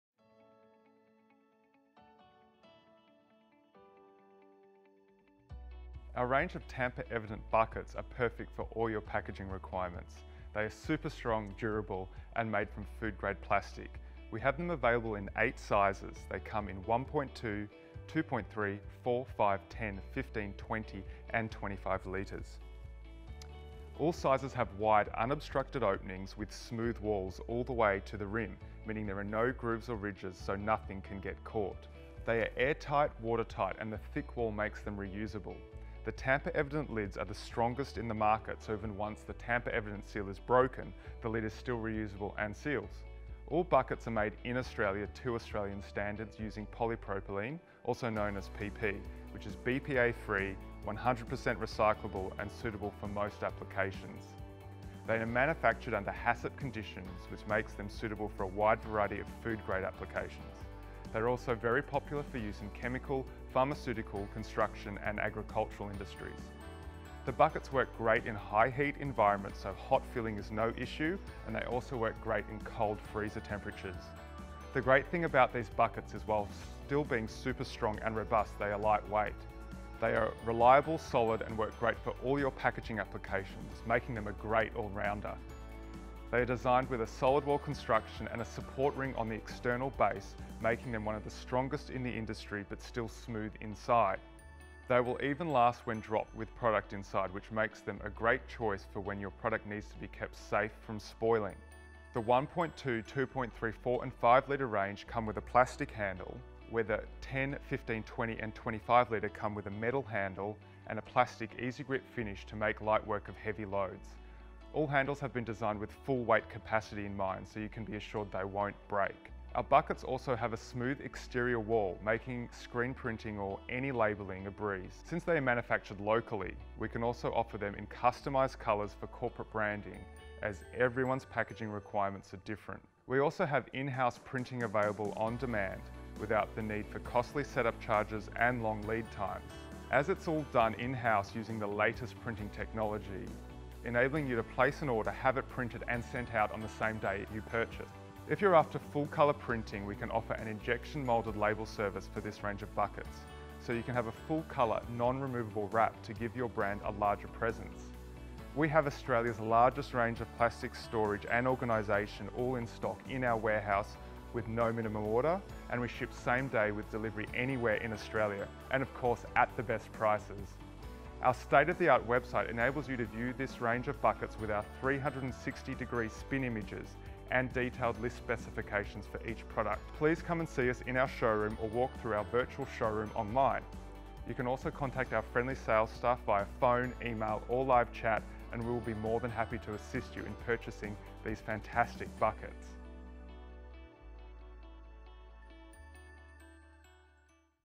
Tamper Evident Buckets
Heavy duty food grade bucket or pail with tamper evident lids. Perfect for anything from packaging food to mixing paints and resins. Comes with strong heavy duty plastic or metal handle so they will not break. This is a commercial food grade buckets for all industry use